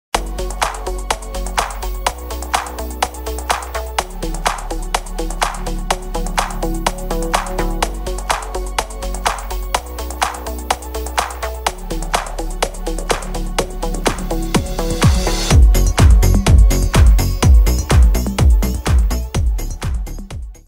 هدف دانجو واتارا ينهي رحلة نسور قرطاج في الكاميرون
عقدة بوركينا لتونس مستمرة.. الخيول أخرجت النسور للمرة الثالثة من أمم إفريقيا  🇧🇫🇹🇳🌍